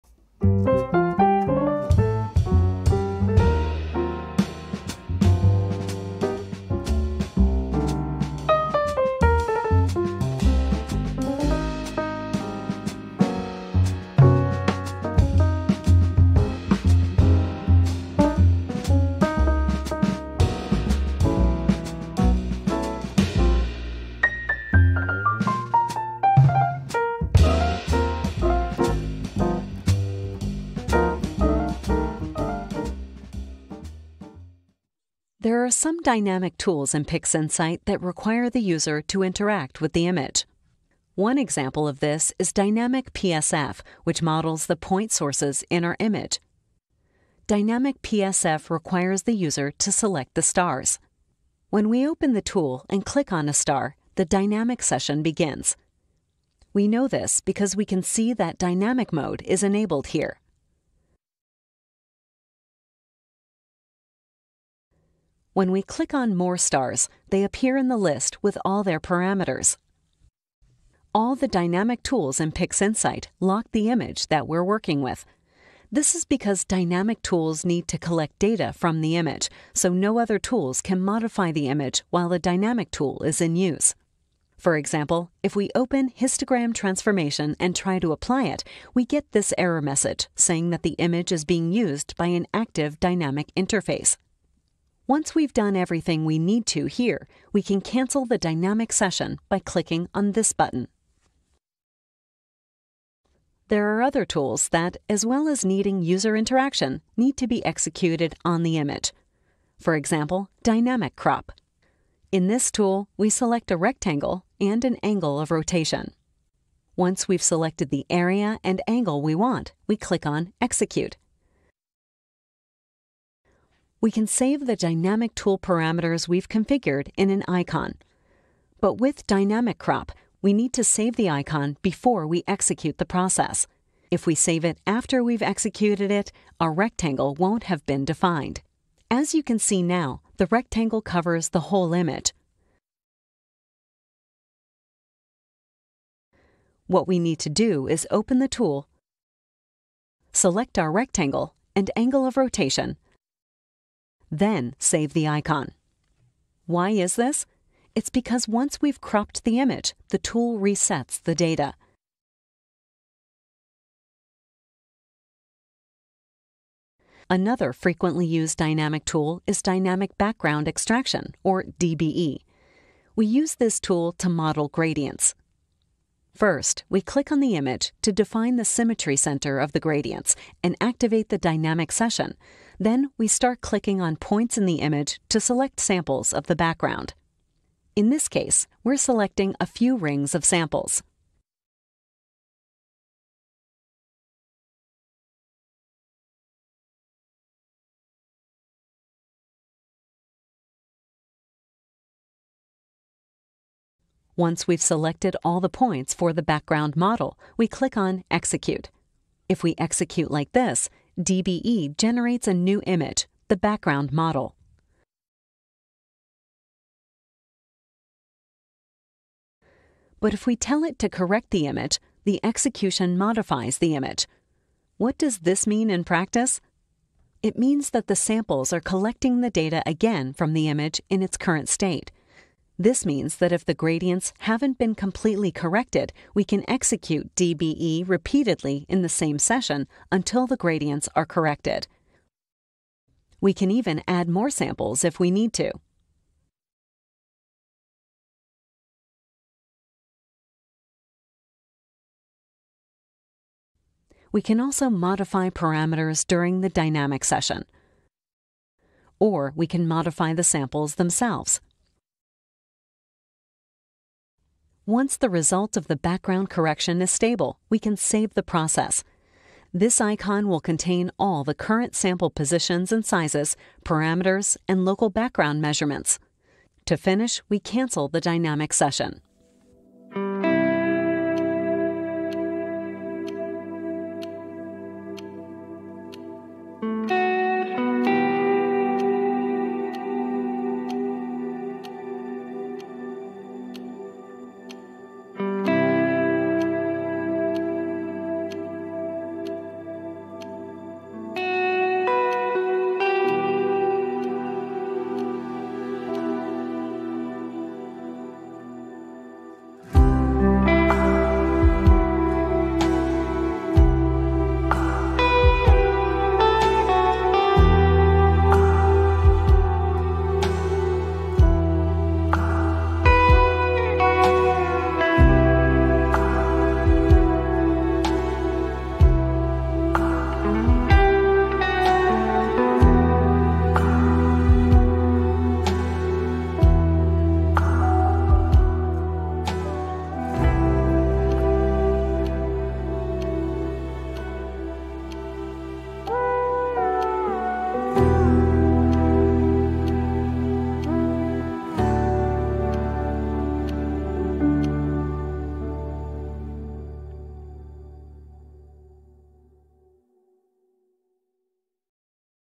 Introduction to PixInsight — 14 Dynamic Processes
Introduction to PixInsight dynamic processes

00:35 Starting a dynamic session
01:42 Saving a dynamic process to an icon
02:44 Executing a dynamic process